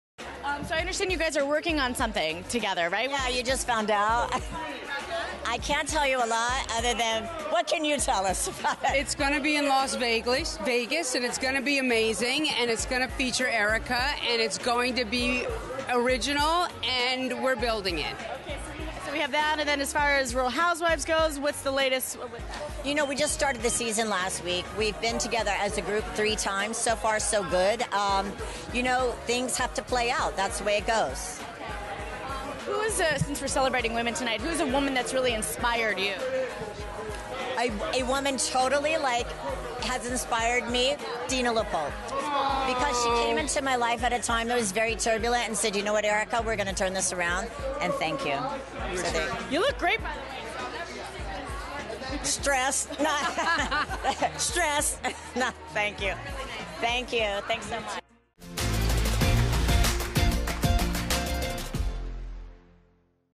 Erika Jayne Talks RHOBH Season 13 at Billboard Women in Music | E! News
Erika Jayne reveals who inspires her most at 2023 Billboard Women in Music Awards. Plus, she teases a new project with friend Dina LaPolt.